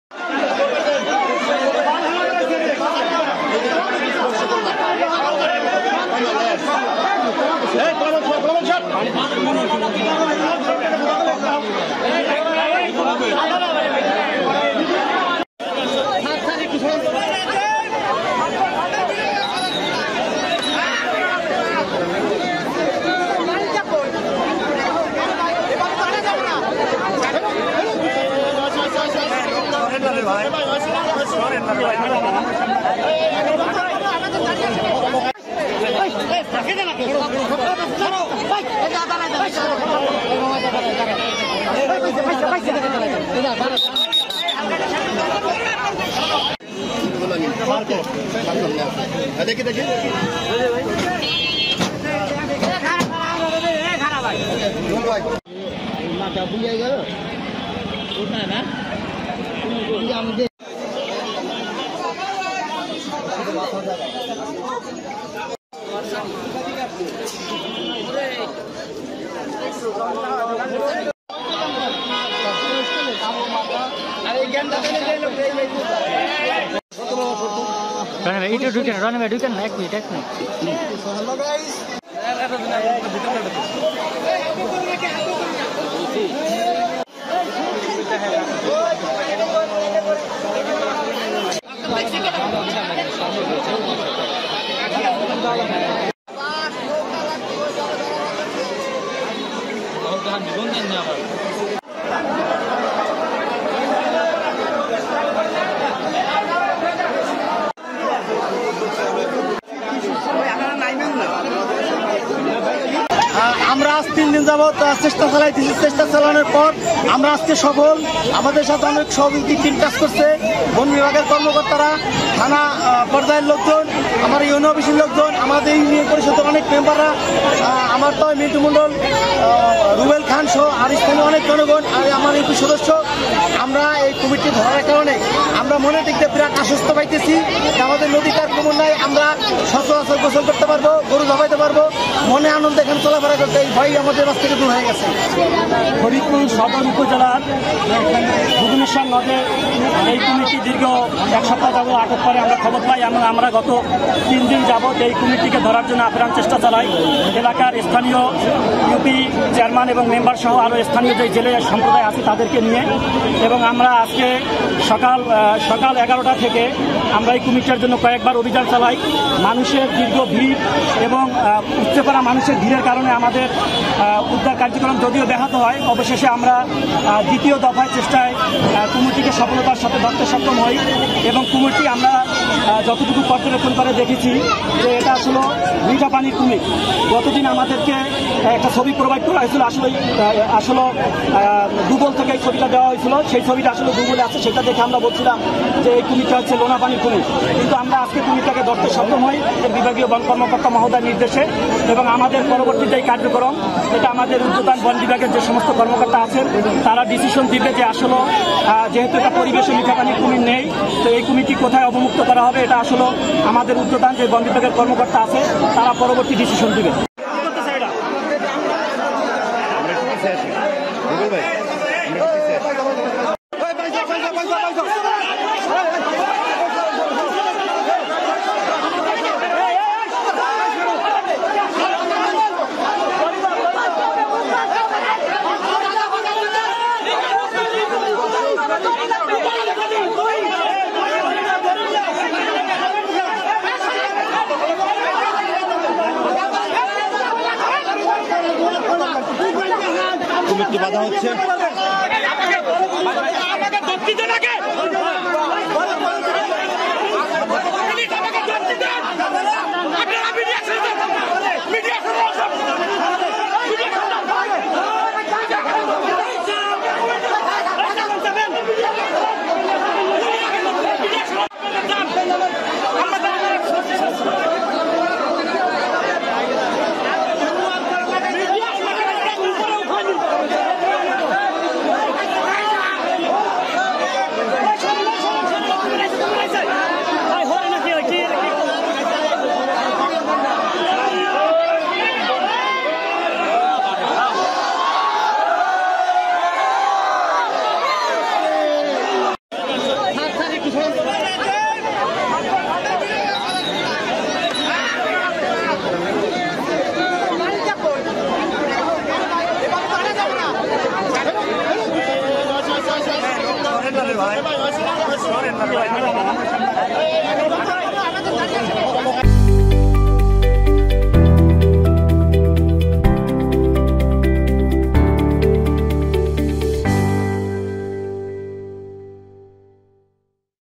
তিনঘণ্টা চেষ্টার পর ফরিদপুরে বিলুপ্তপ্রায় কুমির উদ্ধার | Crocodile | Faridpur | Jamuna TV
Address:
Bangladesh.